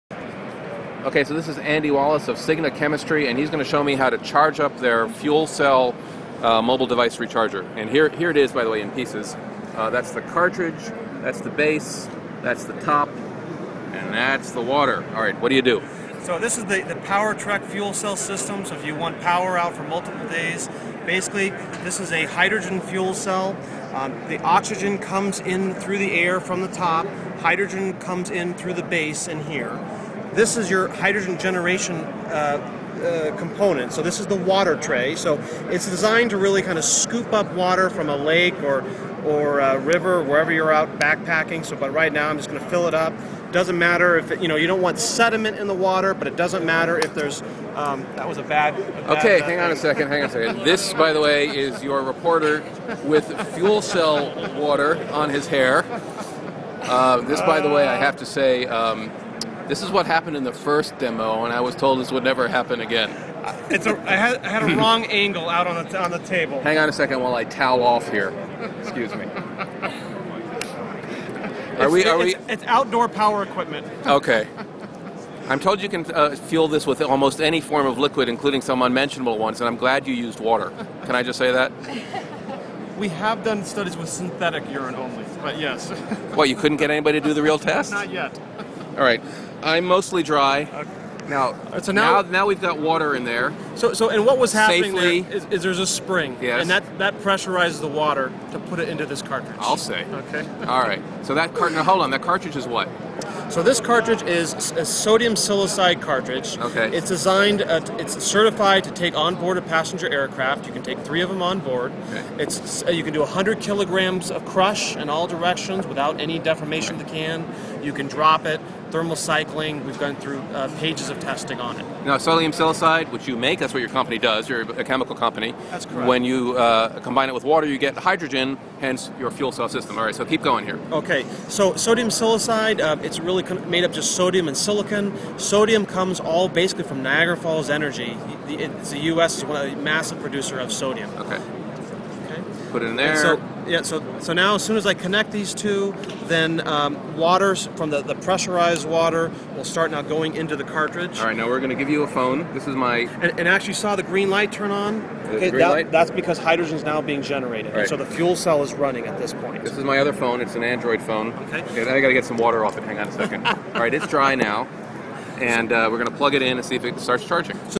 Powertrekk fuel cell recharger makes a splash
During a demo of a new fuel cell recharging system, the water reservoir takes aim at Rafe Needleman, and lets him have it.